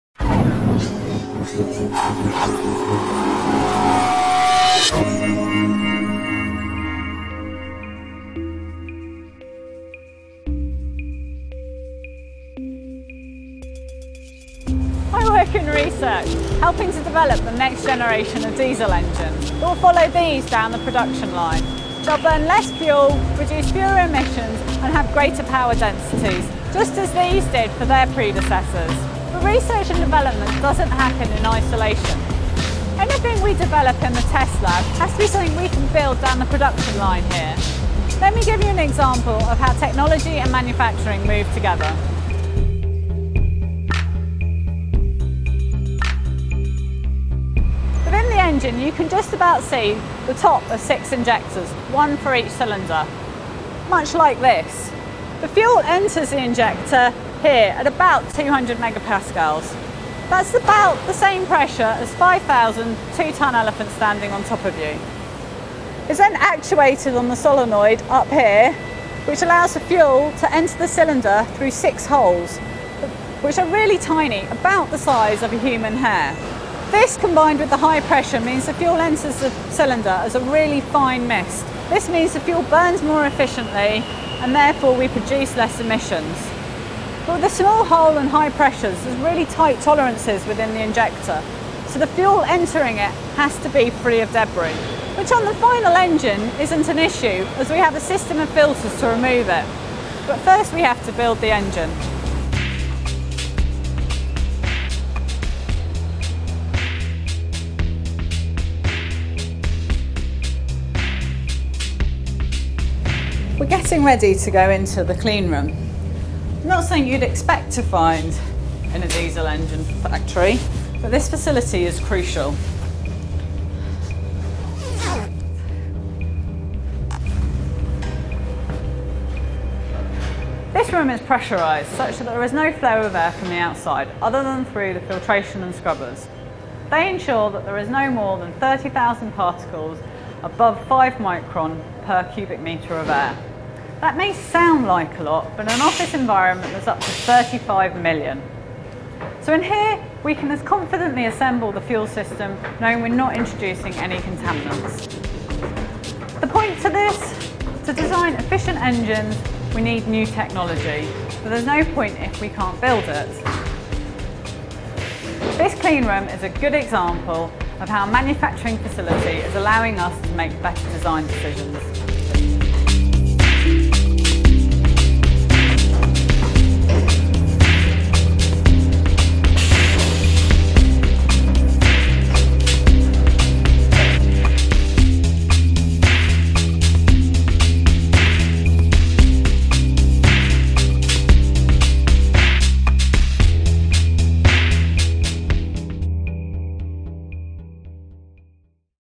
Components: Manufacturing at Caterpillar UK
Research Engineer Hannah Petto takes us behind the scenes at her workplace, the engine test facility of Caterpillar UK in Peterborough.

In this film Hannah reveals how manufacturing and techniology move together in the development of the next generation of high-power diesel engines.

This film is part of a project funded by the Royal Academy of Engineering to develop the on camera  communication skills of engineers across the UK